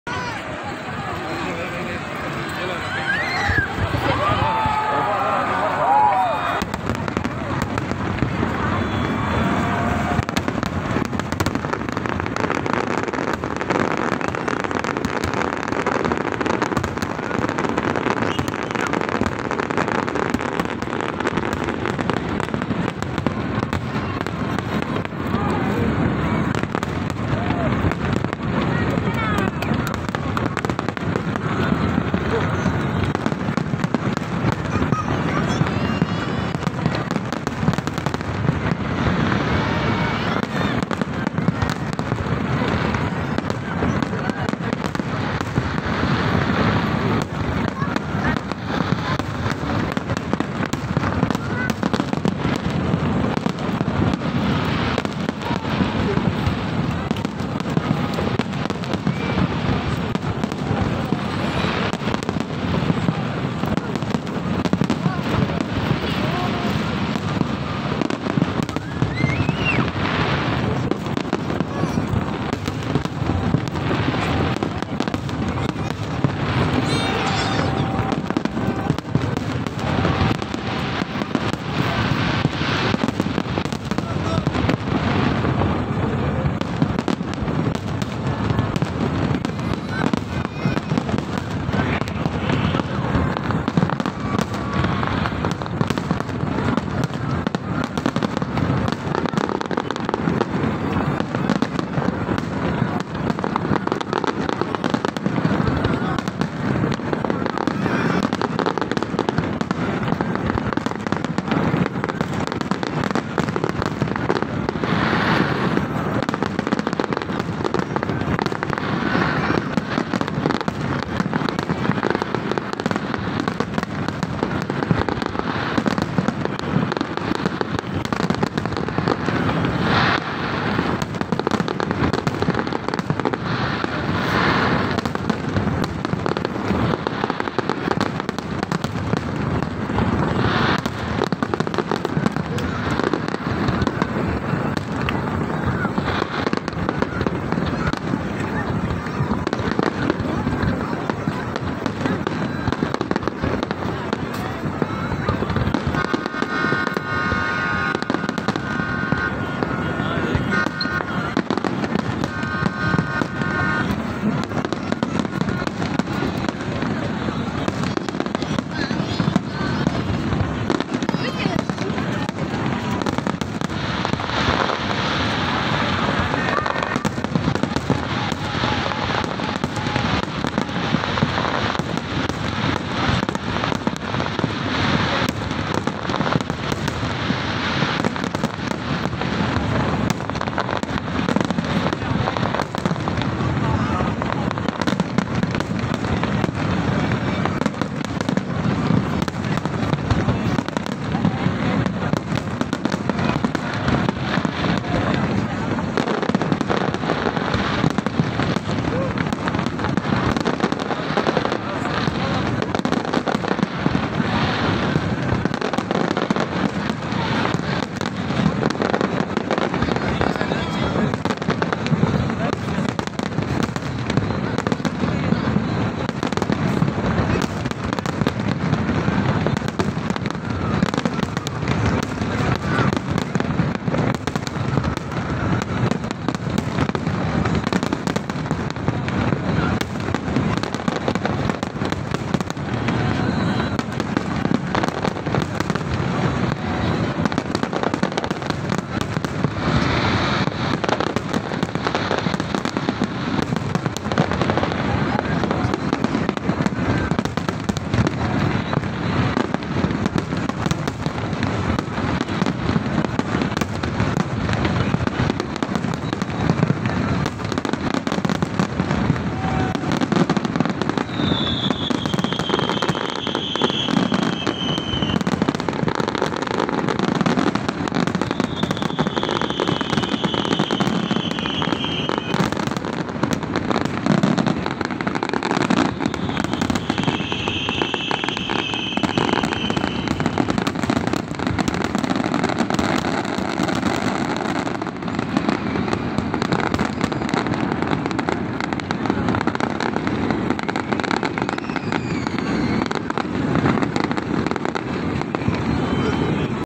Fire Works Abu Dhabi Marina corniche | Abu Dhabi fire works 2021 | Happy new year fireworks
in this Video you can see Beautiful Fireworks at Abu Dhabi Marina Corniche  UAE